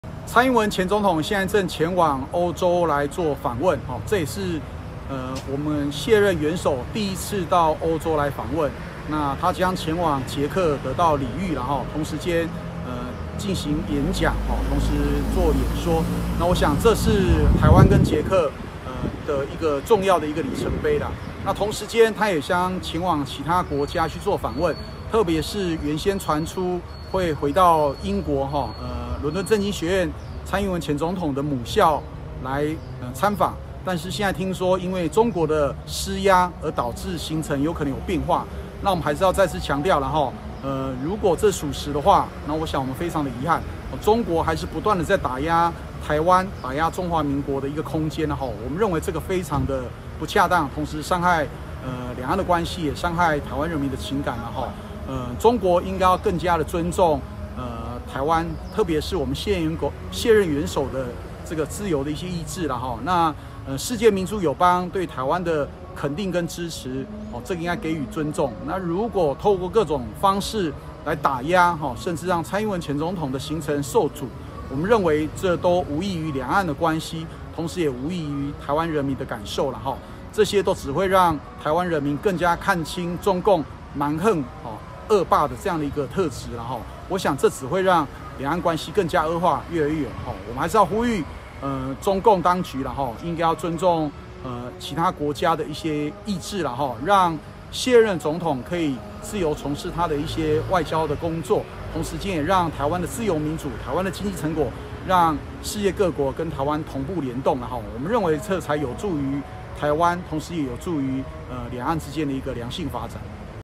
快新聞／又打壓台灣！蔡英文訪英行程因中國施壓生變 綠委怒轟：蠻橫惡霸－民視新聞
【民視快新聞】即時中心／高睿鴻報導

台灣前總統蔡英文近日展開訪歐之旅，但卻傳出插曲，據英國媒體《衛報》披露，英國外交部傳出希望台灣推遲蔡英文訪英，避免影響外交大臣拉米（David Lammy）訪問中國的行程。對此，民進黨立委賴瑞隆今（13）天將矛頭對準中國，直言說，倘若傳言為真，即可證明中國直至今日，依舊持續打壓台灣、壓迫中華民國生存空間；他認為此舉相當不恰當，不僅再次破壞兩岸關係、也傷害了台灣人民的情感。